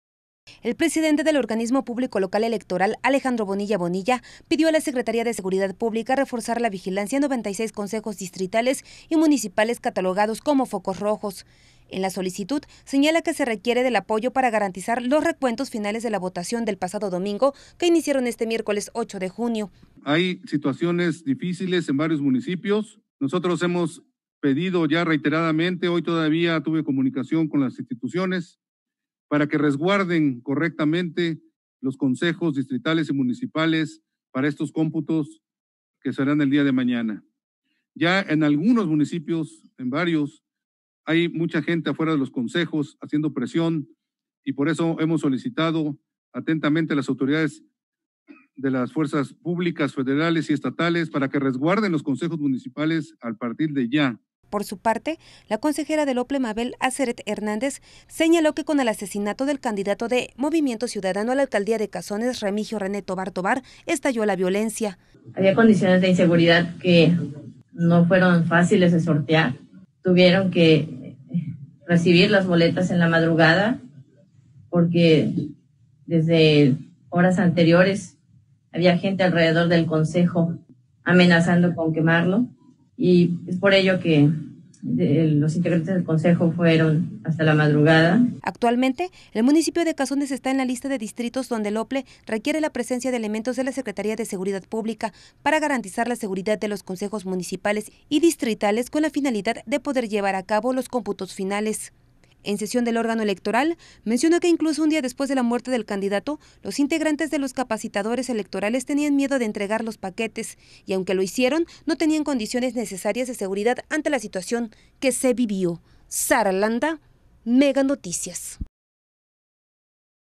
El OPLE pidió seguridad para 96 consejos distritales
El presidente del Organismo Público Local Electoral (OPLE), Alejandro Bonilla Bonilla, pidió a la Secretaría de Seguridad Pública (SSP) reforzar la vigilancia en 96 consejos distritales y municipales catalogados como "focos rojos".

En la solicitud, señala que se requiere del apoyo para garantizar los recuentos finales de la votación del pasado domingo, que iniciaron este miércoles 8 de junio.